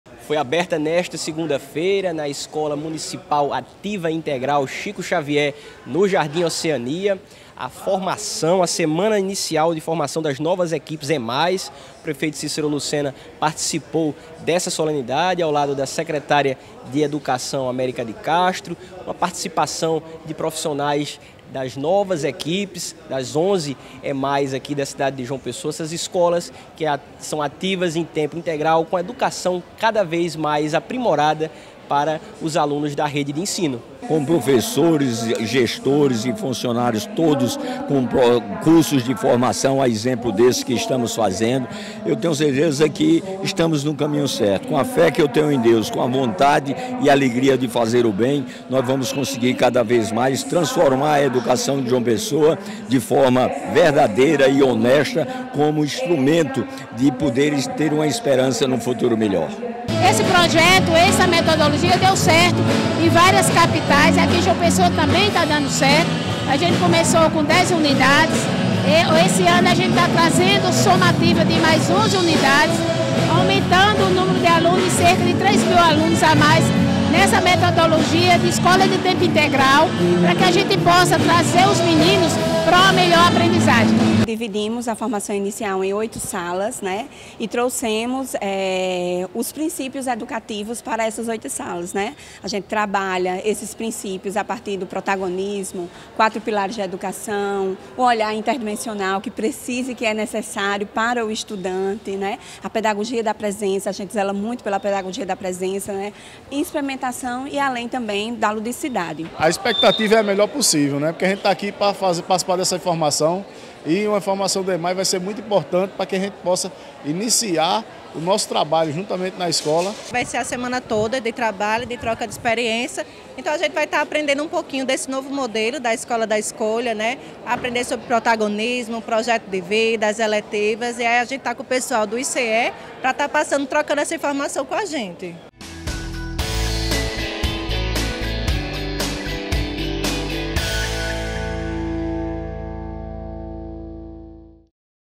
DIA A DIA NA CIDADE - ABERTURA FORMAÇÃO SEDEC
A Prefeitura de João Pessoa abriu nesta segunda-feira (19), a formação das novas equipes EMAIs (Escola Municipal Ativa Integral).

🔴 Confira como foi a solenidade na Escola Chico Xavier na reportagem de Daniel Lima.

▶️👆 Dá o play‼️